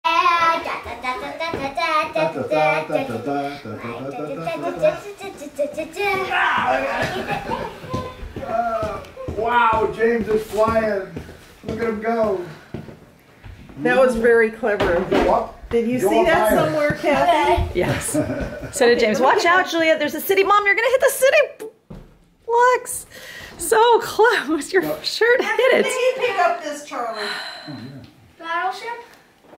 Julia and James Show Papa and Gaga Their Halloween Costumes
Taken 11/12/17. Julia and James show off their Halloween costumes. Papa tries to catch Julia's octopus leg. Gaga nearly knocks over James's tower of Citi Blocks.